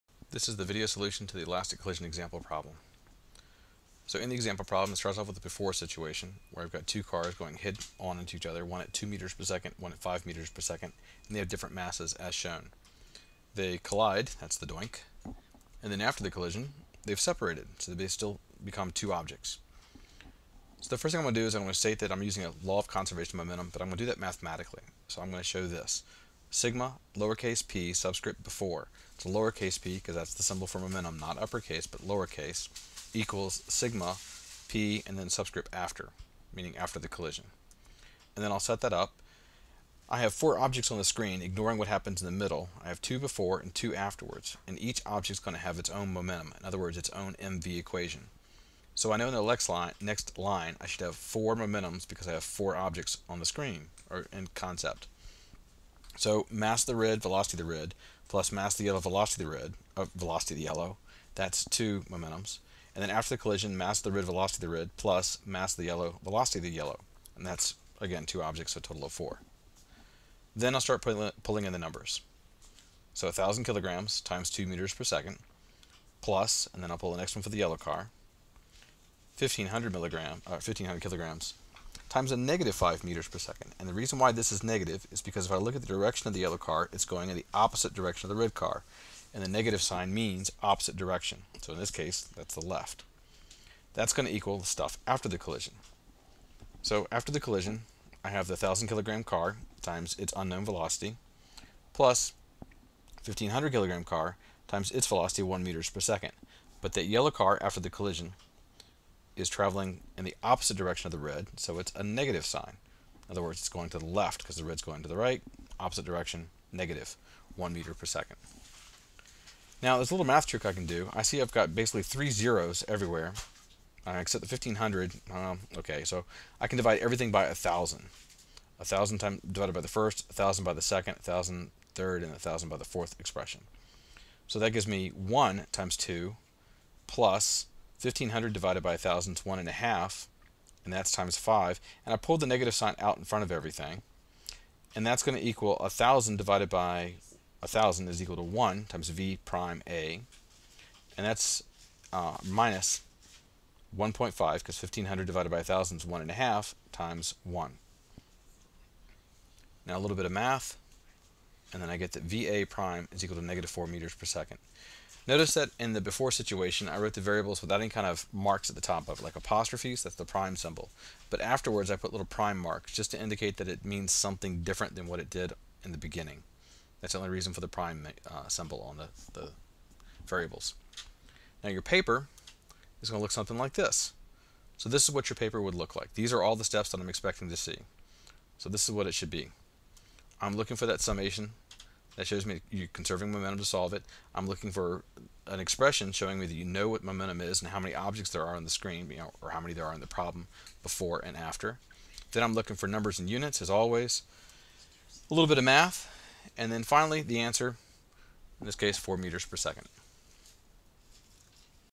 Elastic Collision Example Problem
They is the solution to an example problem using the law of conservation of momentum for a perfectly elastic collision.